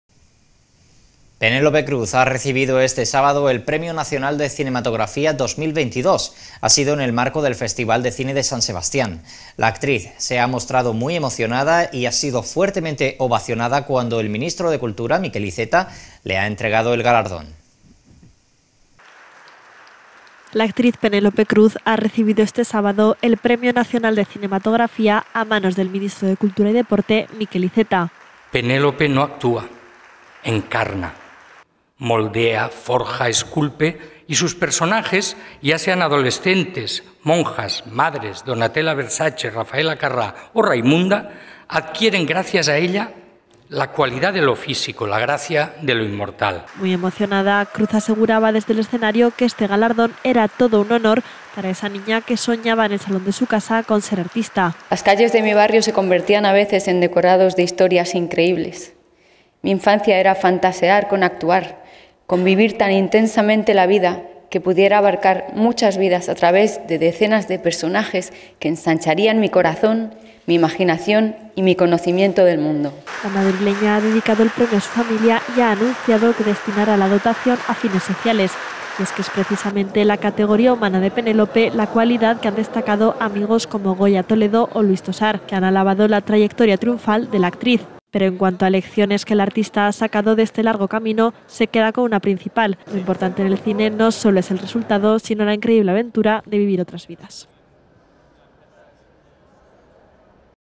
🔴Noticia - Penélope Cruz, Premio Nacional de Cinematografía 2022
La actriz Penélope Cruz ha recibido este sábado el Premio Nacional de Cinematografía a manos del ministro de cultura y Deporte Miquel Iceta. Muy emocionada, Cruz, aseguraba desde el escenario que este galardón era todo un honor para esa niña que soñaba en el salón de su casa con ser artista.

English:

The actress Penélope Cruz has received this Saturday the National Cinematography Award at the hands of the Minister of Culture and Sports Miquel Iceta. Very excited, Cruz assured from the stage that this award was an honor for that girl who dreamed of being an artist in her living room.

Live streaming